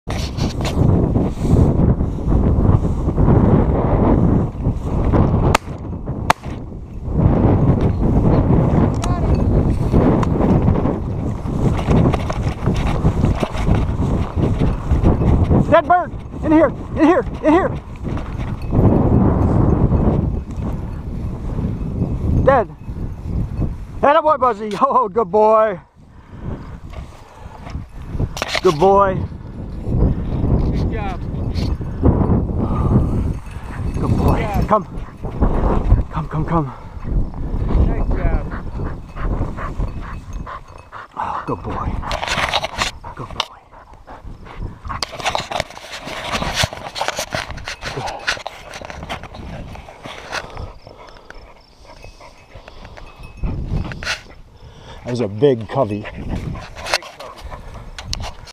montana huns 2022
nice opening week covey of huns in eastern montana.  my cocker buzz does a nice job with them